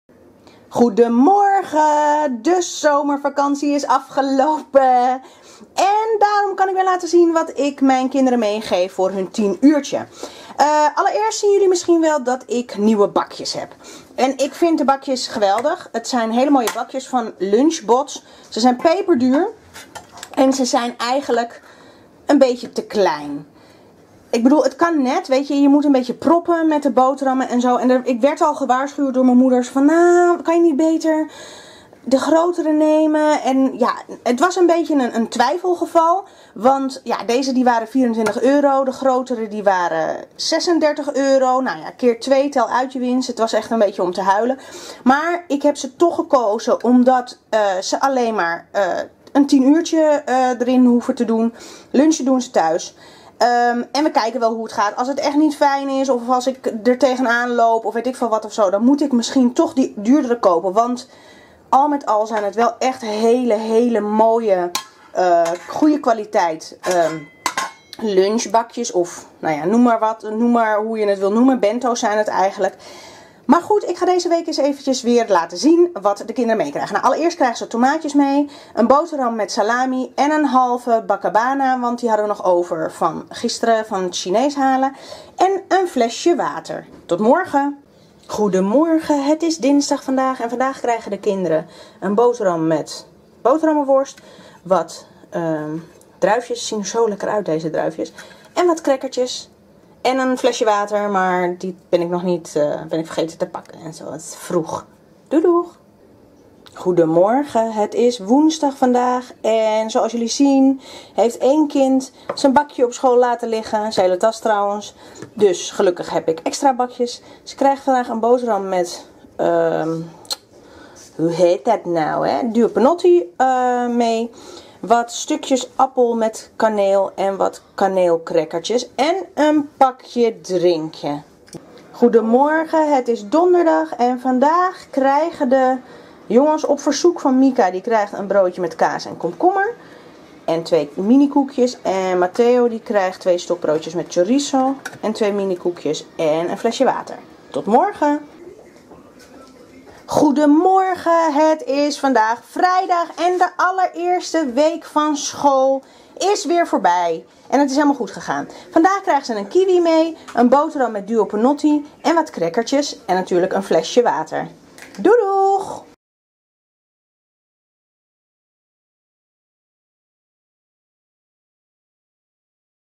Wat krijgen de kids mee naar school, 28 aug tot 2 sept!
Dit is wat ik mijn kinderen mee naar school geef voor hun 10-uurtje.
Lunchen doen ze thuis.